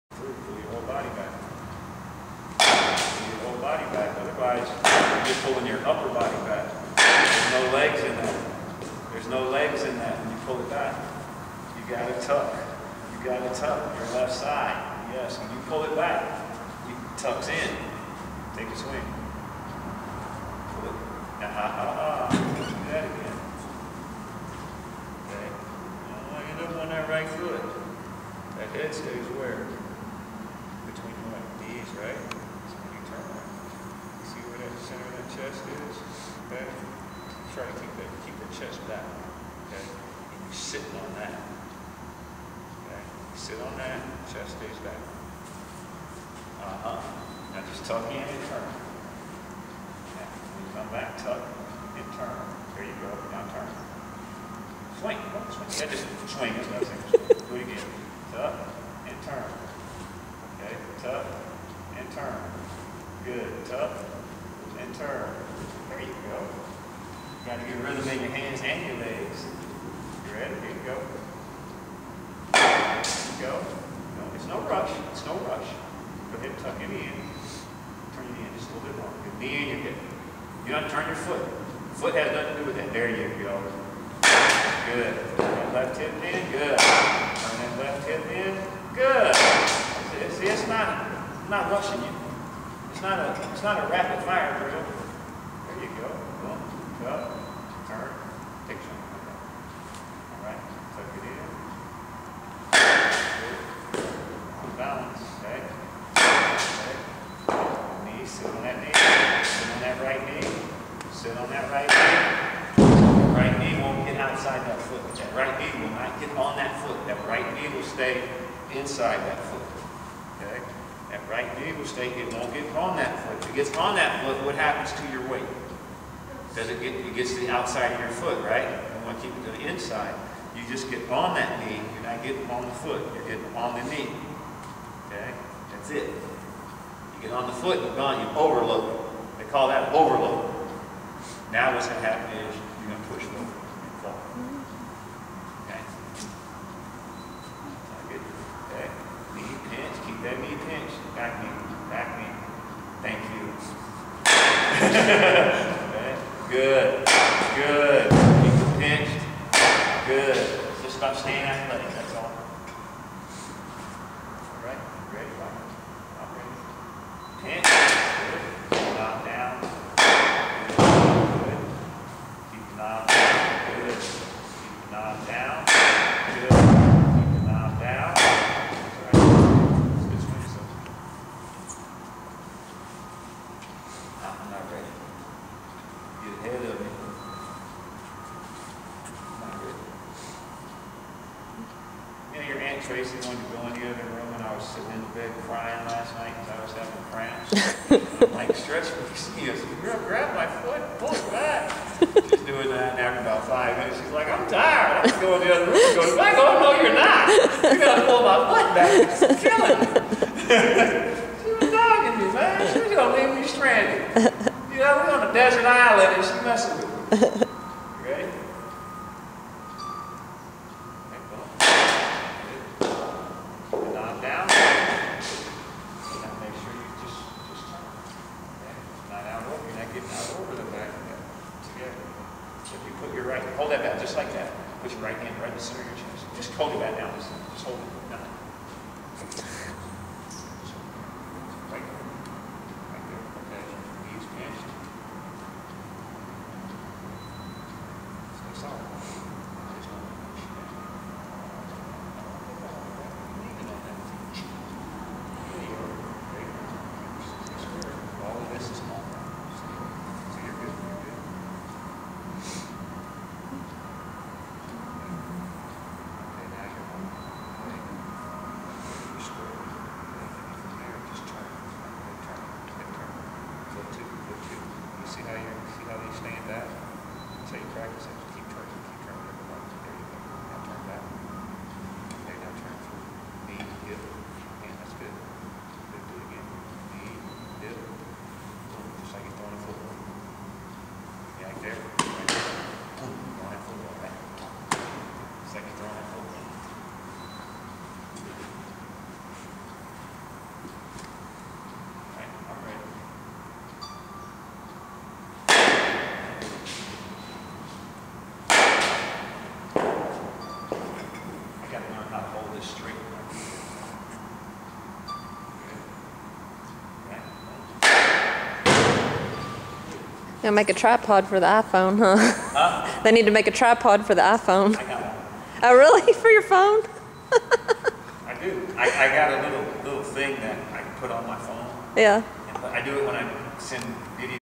Cecil Espy hitting lessons: Alex Lee
Alex taking hitting lessons from his uncle, Cecil Espy